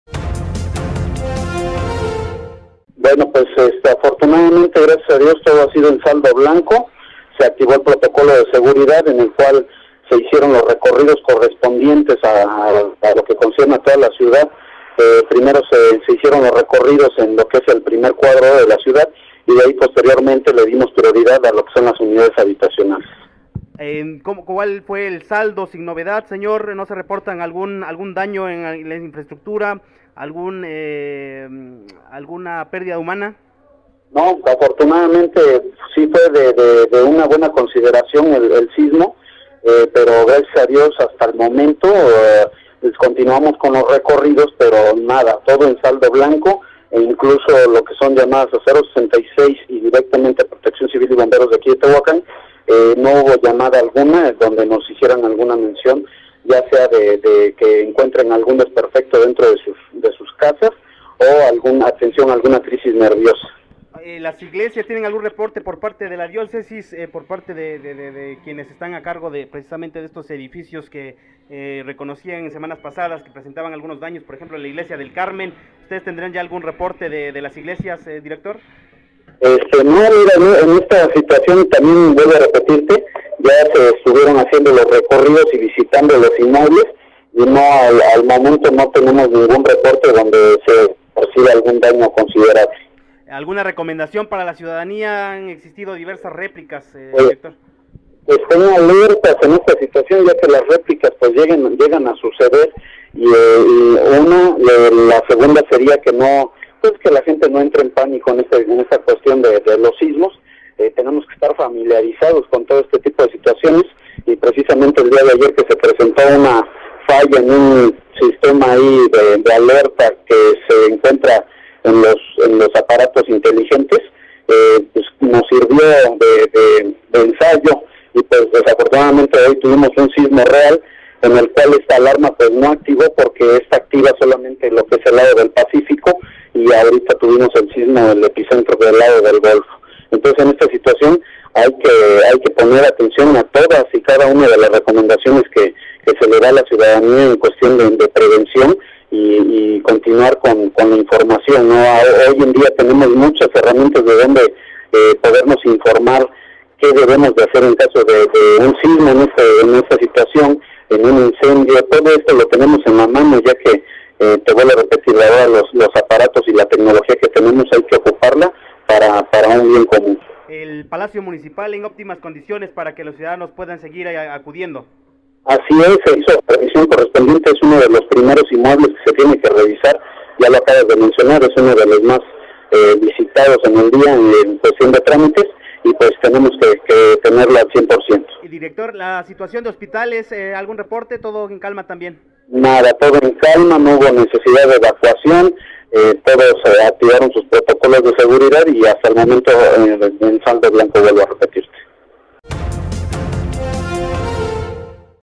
Sismo no provocó daños en Tehuacán
Reporta Protección Civil de Tehuacán